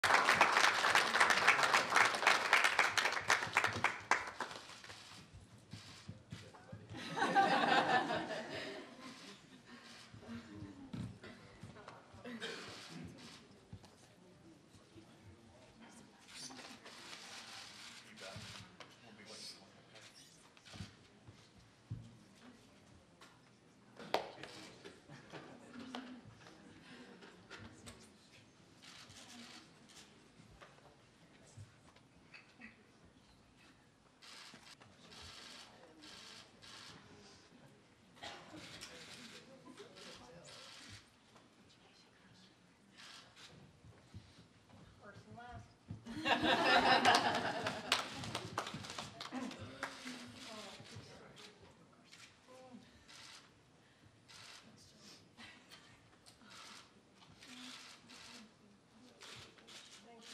EBOLA PATIENTS-BRANTLY: HUGS EVERYONE
Ebola patients released from Emory hospital.